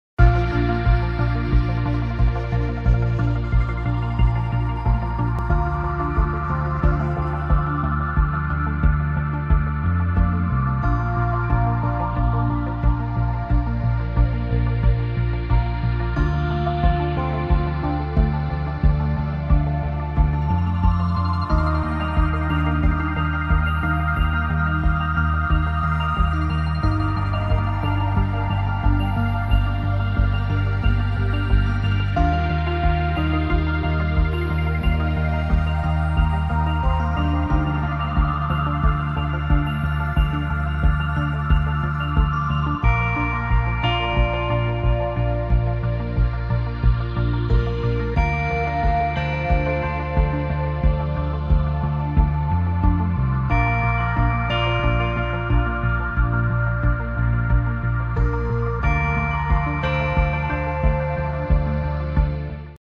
Les religions unies...dans un pays arabe ! 😍🕌🕍⛪
Une mosquée, une église et une synagogue sur un seul et même site dans un pays arabe ! 😍🕌🕍⛪